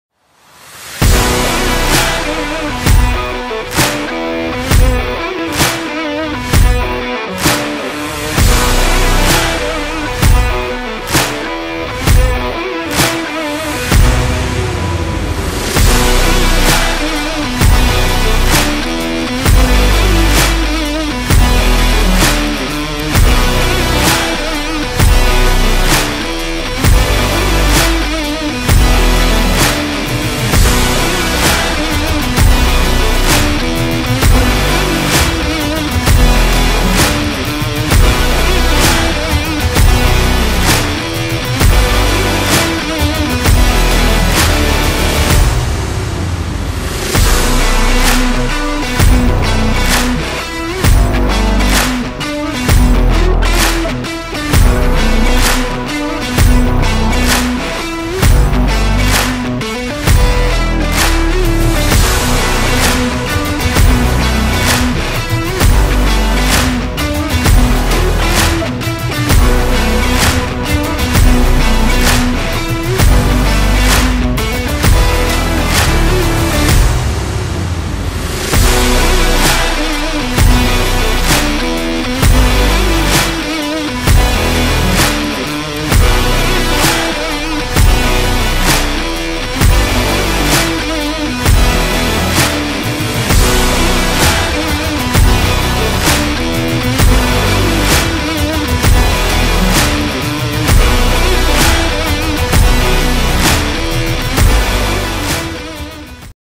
Mazenodian Vehical Parade 2023
2023 Sport Feista in De Mazenod College at Kandana
School Vehicle Parade.